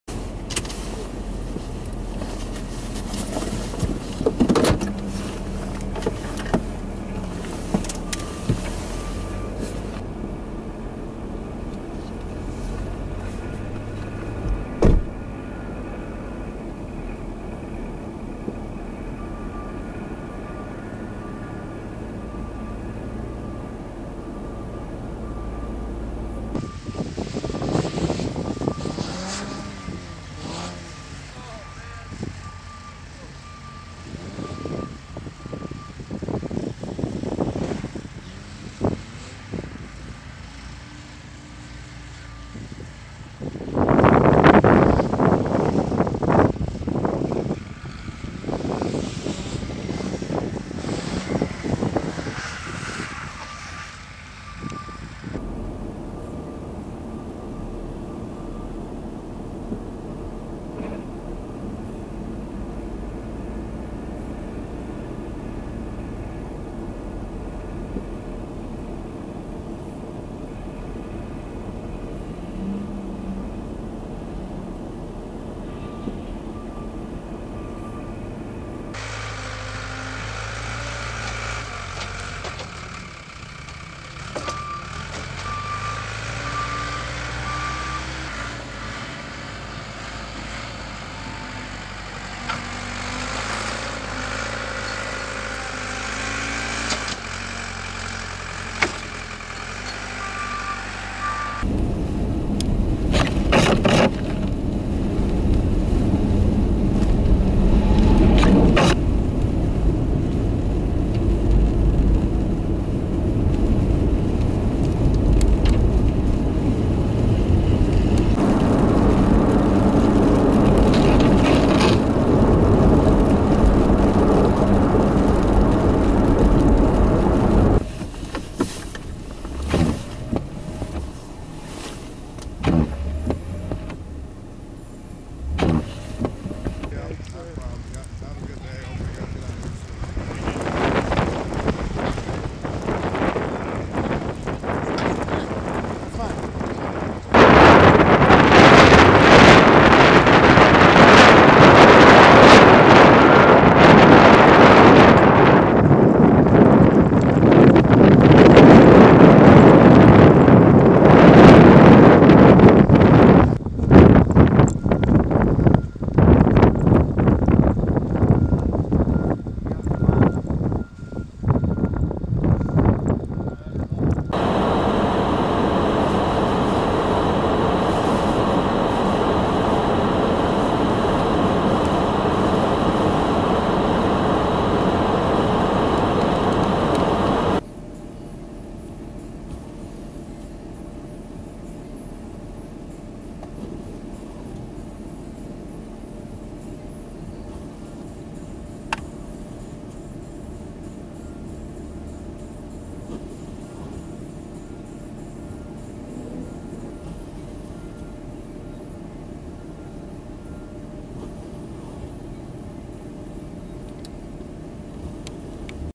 02-24-2017-Algona-Iowa-Morning-Cars-Stuck
Algona, Iowa was trying to recover from yesterday's blizzard and large snow totals. Cars were getting stuck on the side roads.

Shot 1-2. Wide and close shot of two men trying to get a car out of the snowbank

Shot 3. Close shot of spinning tires on a stock car in a snowbank

Shot 4-5. Wide and different angles of a man pushing a car out of the snowbank

Shot 6-7. Medium and close shots of a tractor plowing snow in a parking lot

Shot 8-9. Driving shots of a plow and a tractor plowing Highway 18

Shot 10. Driving with minimal visibility south on Highway 18

Shot 11. Medium shot of a car stuck in the snow

Shot 12-14. Panning shots in a medium shots of a truck plow being pulled out of the snow after trying to help a car that was stuck

Shot 15. Close shot of the snow and the car high centered that was stuck

Shot 16. Medium shot of a tractor moving snow so the car can turn onto Highway 18

Shot 17-19. Medium and close shots of spinning tires on the minivan and a man shoveling the car out of the snow